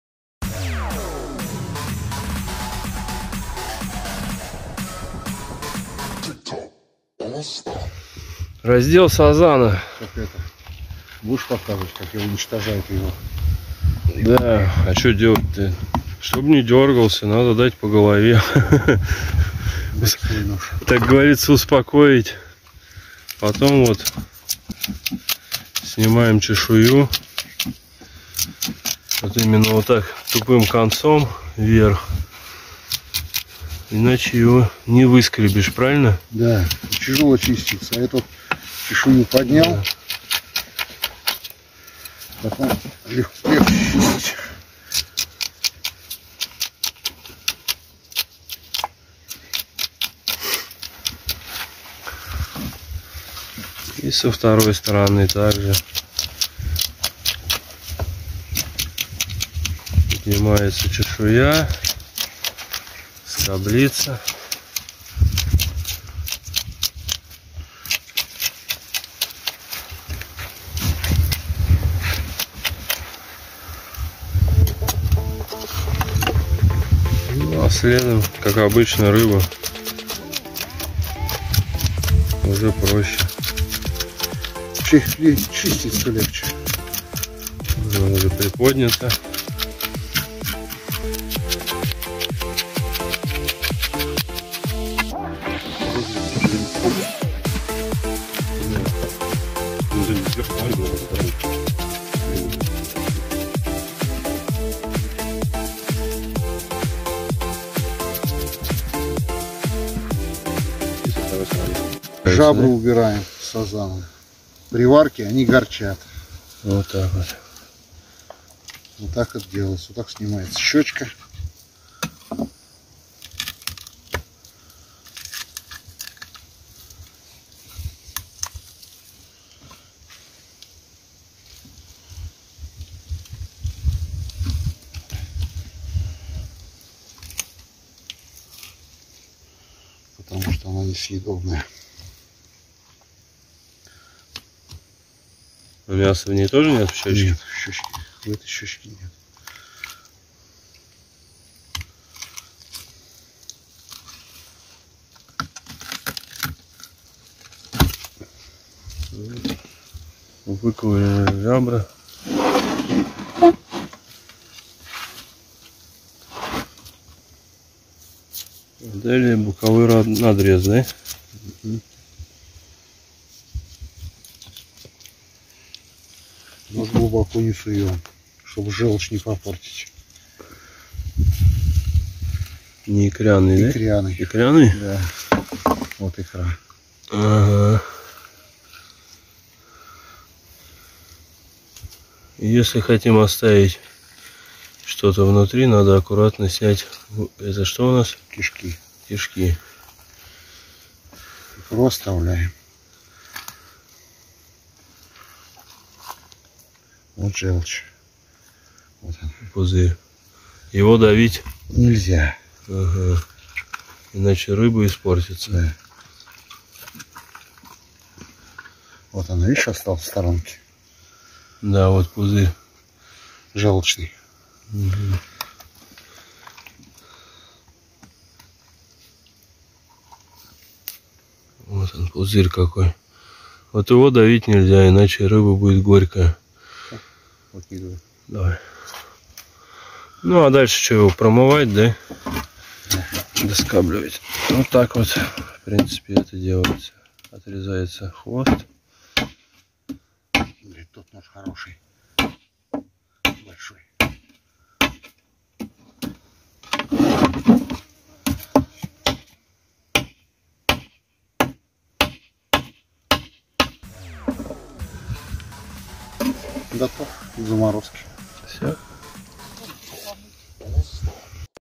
Как почистить САЗАНА? Легко снять чешую. Не попортить внутренности,чтобы не горчил
Как почистить сазана.Правильно снять чешую.Вытащить внутренности ,чтобы не испортить рыбу.